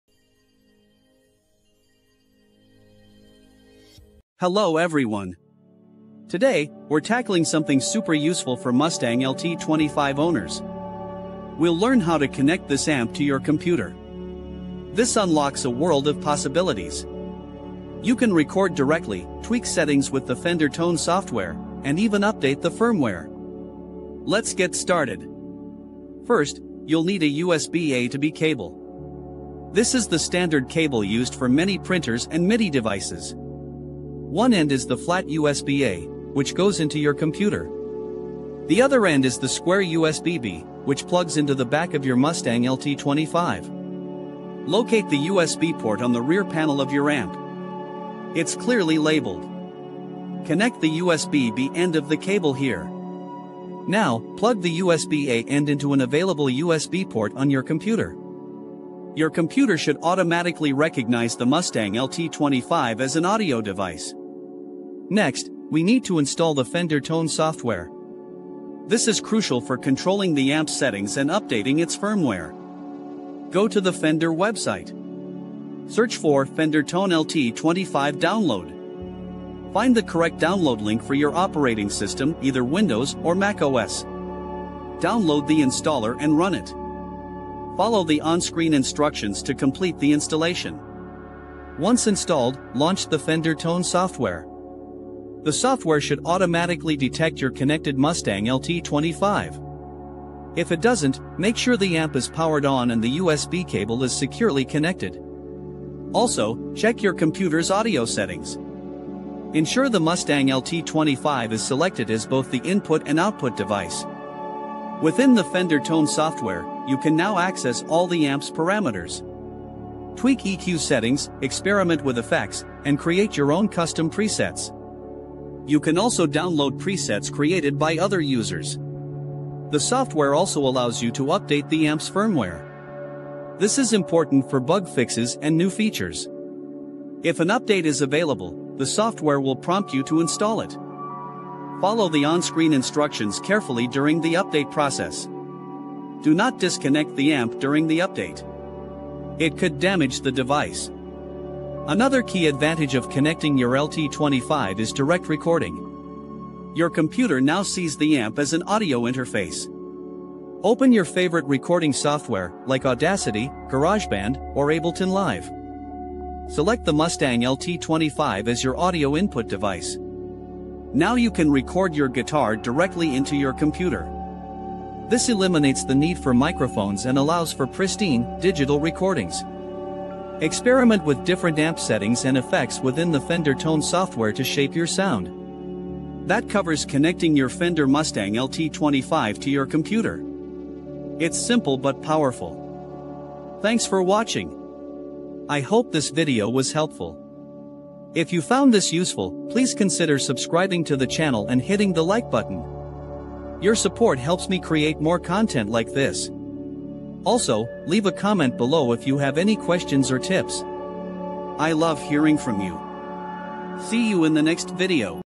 How to Connect the Fender Mustang LT25 to Your Computer
Connecting your Fender Mustang LT25 to your computer allows you to access Fender Tone LT, a software platform for editing presets, recording, and updating firmware. The connection is simple: use a standard USB-A to USB-B cable (the kind commonly used for printers). Plug the USB-A end into a USB port on your computer and the USB-B end into the USB port located on the rear panel of the Mustang LT25. Once connected, your computer should recognize the amp. You may need to install Fender Tone LT software from the Fender website if you haven't already. After installation, launch the software. It should detect the amp, allowing you to begin editing, recording, or updating your amp's firmware as needed.